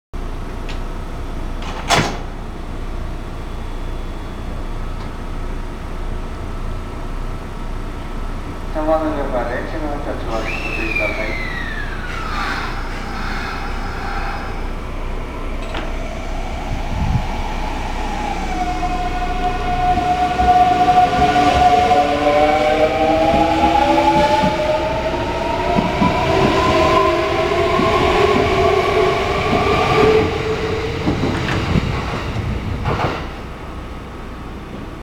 近鉄特急 奈良線を走るアーバンライナーnext＠生駒駅出発
大阪難波発奈良行き特急＠生駒駅にて